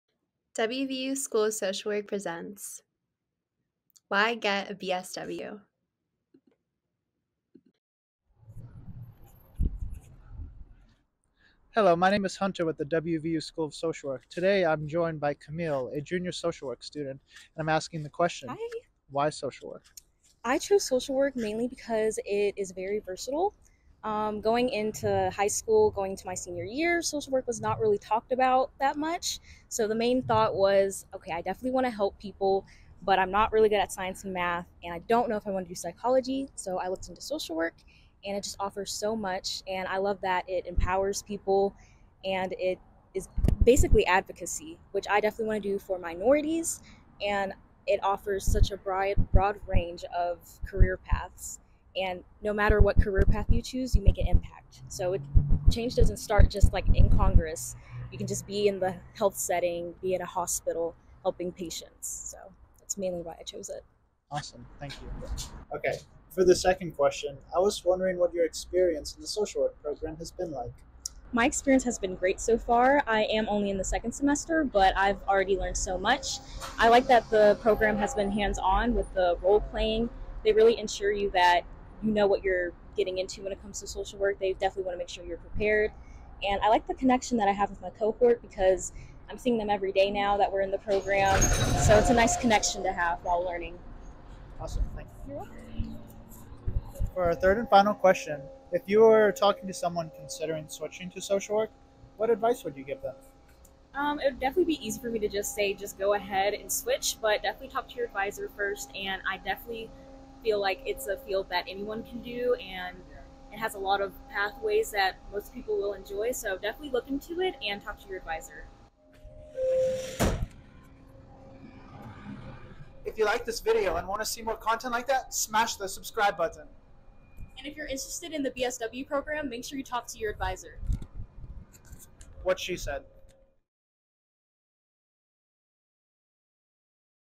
WVU School of Social Work: Why Get a BSW?
Listen to current WVU BSW Student, Kamille Moore, speak about her experience in the program. She highlights why she chose social work and advice to those interested in switching their major.